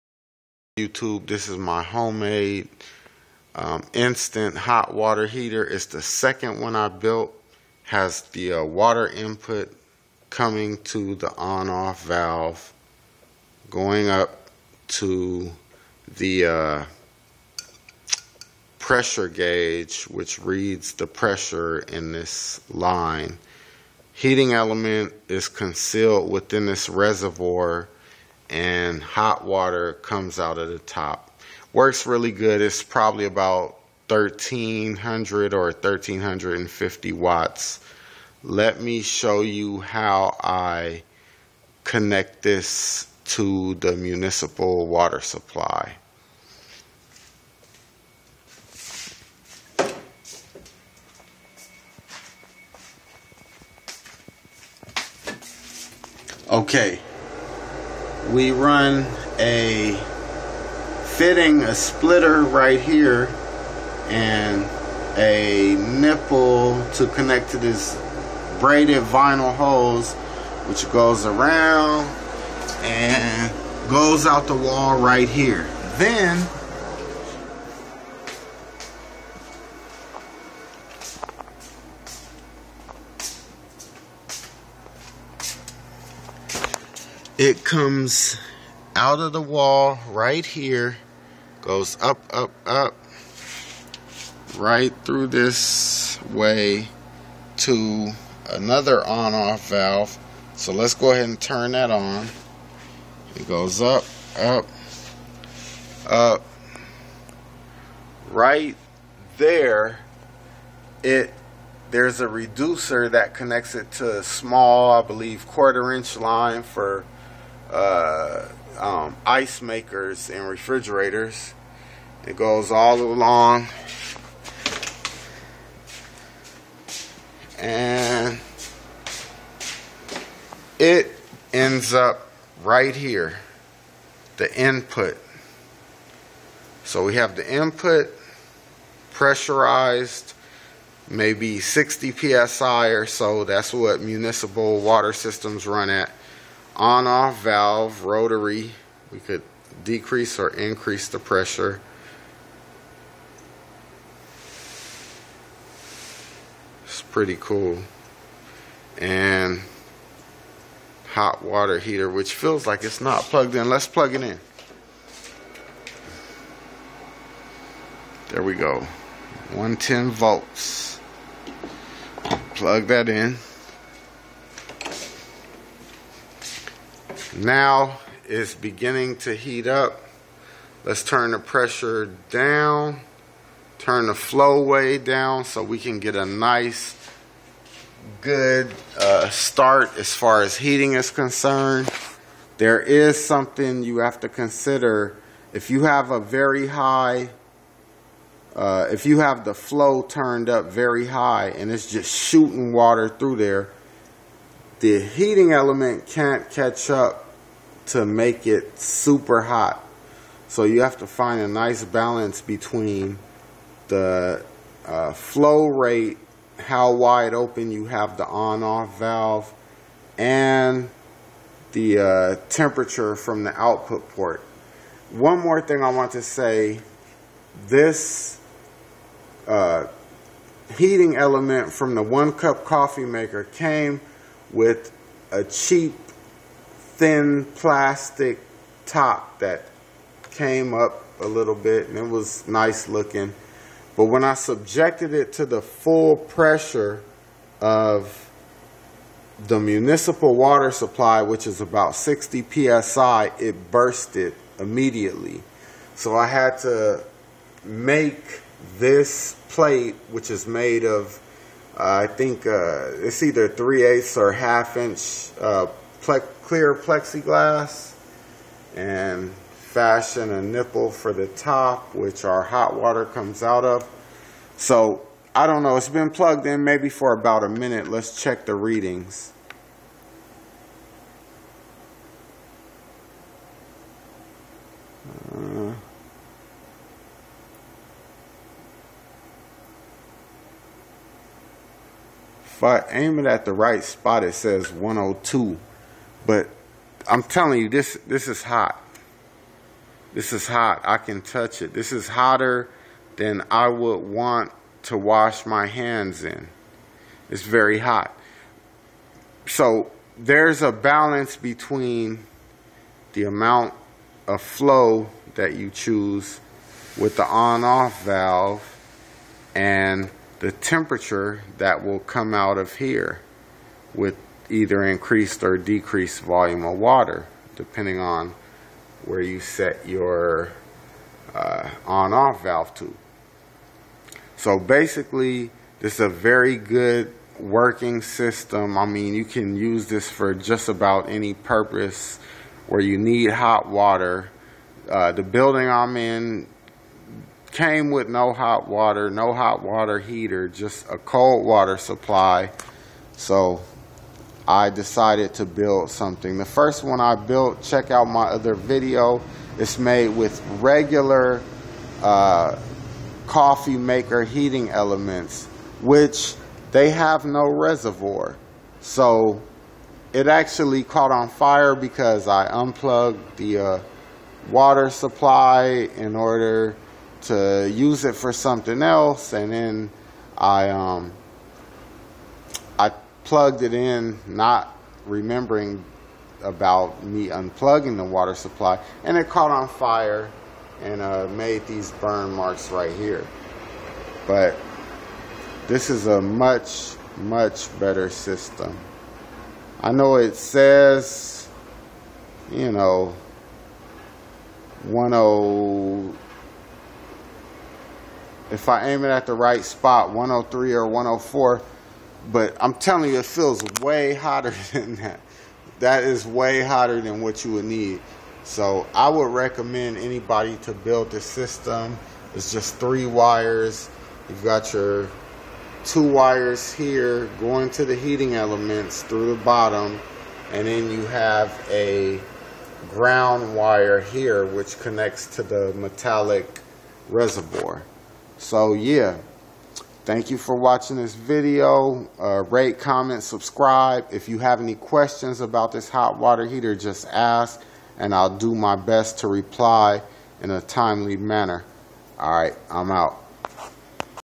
One Cup Coffee Maker To Instant Hot Water Heater, Custom Setup.
I made my second 110 volt instant hot water heater using the heating element from a one cup coffee maker which comes in its own reservoir. It gets extremely hot with the water input at a minimum flow. Its much safer than my first water heater (see my other video) which caught on fire. If the first 110 volt water heater had a reservoir, It would have made a lot more steamy type noise and I could have unplugged the cord before it melted!